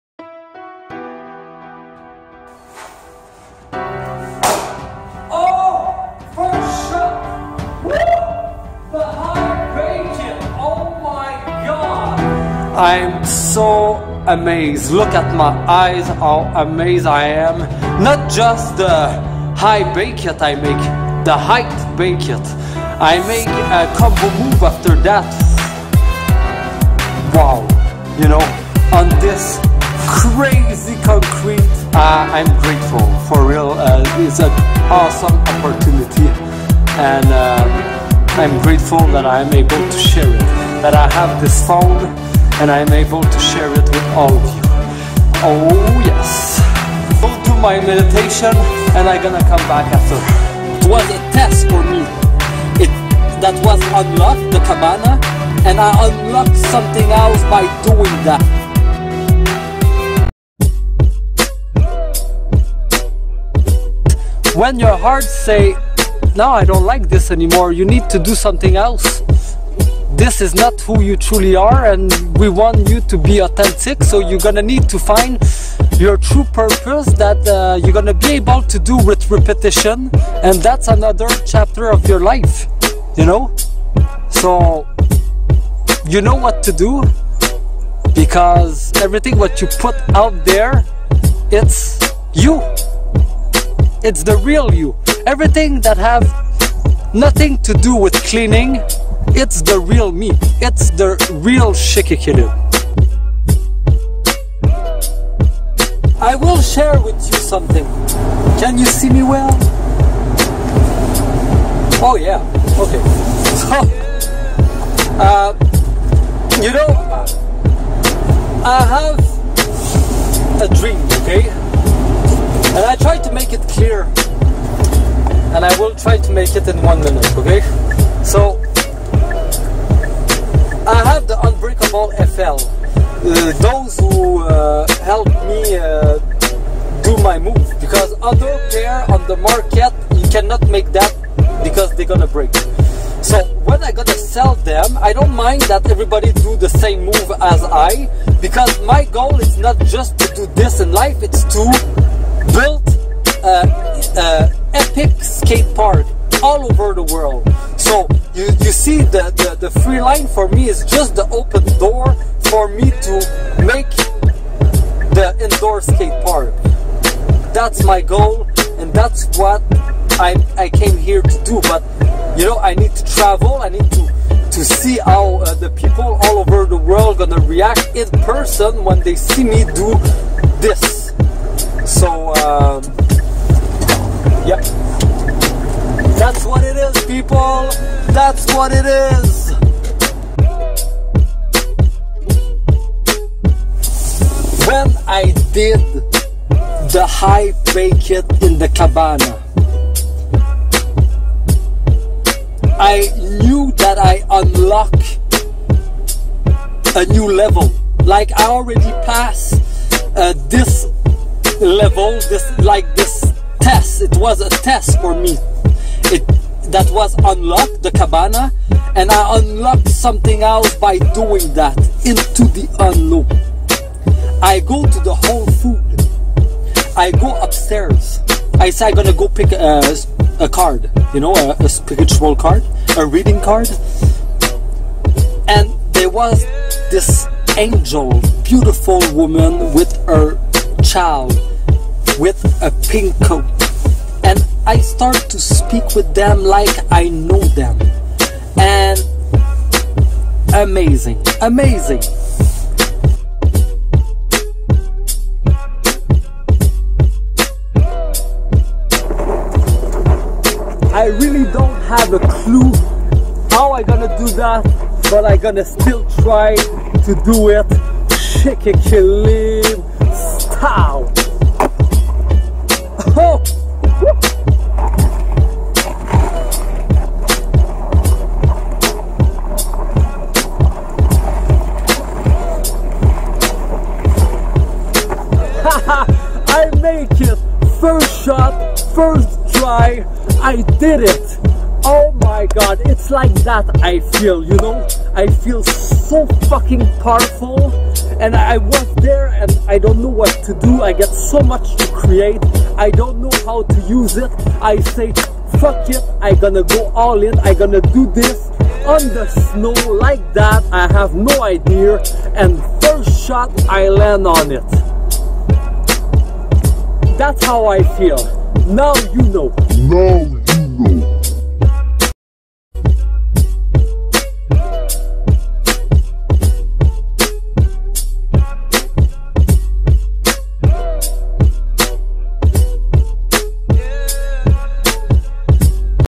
💫Chic 🦄Unicorn🎦 Reveal 2 - 🏚Cabana👆level Up☝ ( Part 7 - bonus Part 8 )
🔥This is the final part of eps 2 and its Definitely its a blessing that I found this cabana in the middle of a snow storm at -30 and its because of my curiosity and my lack of being afraid that make that amazing freeline epic performance on that  slippery concrete in the cold but still protected with roof  and walls. After did the  height bake it I feel that i unlock a new level and i did after that!!! The bonus part is a preview of that new level and its  gonna be chic unicorn reveal eps 3. Enjoy😊